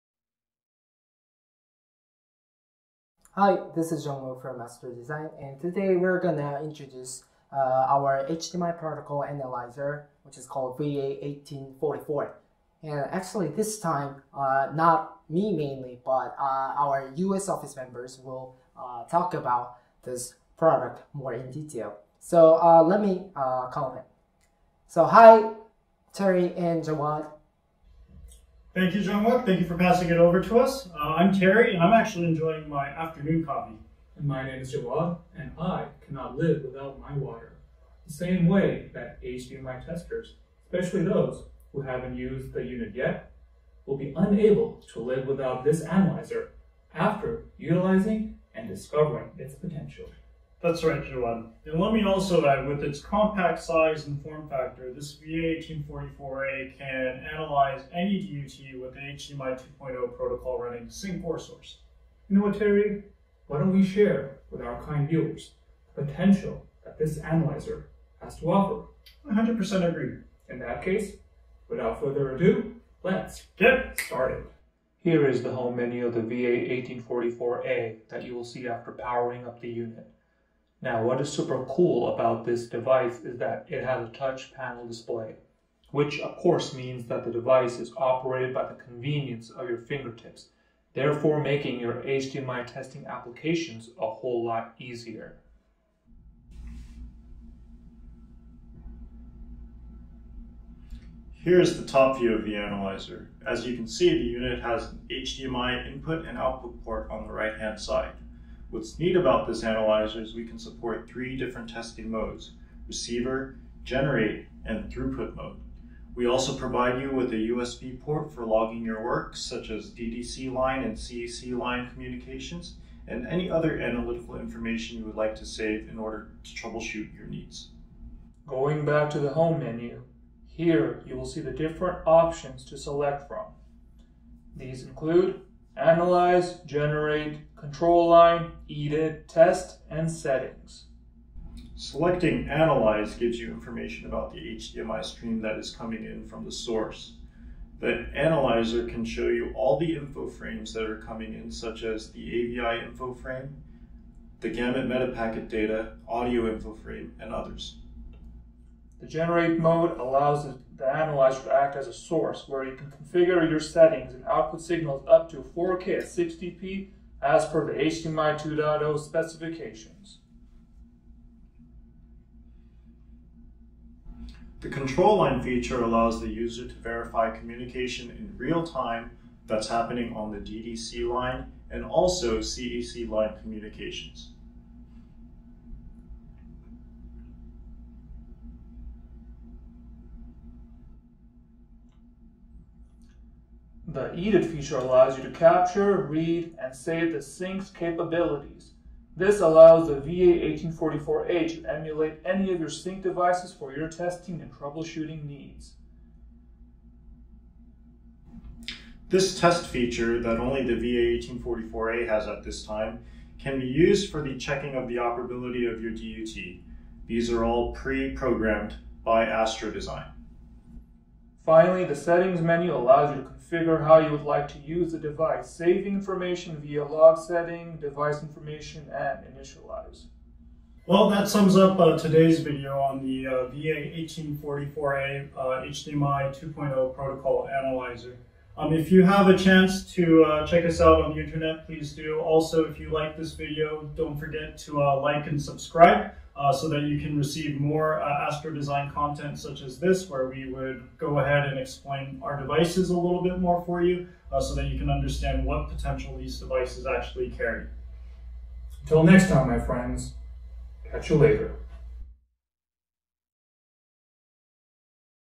ASTRODESIGN, Inc. Product PR Video - HDMI 2.0 Portable Analyzer VA-1844A (Part 1. General Overview)
In this video, we introduce the outline and overall functions of the VA-1844A, a compact HDMI 2.0 protocol analyzer that supports up to 4K resolution.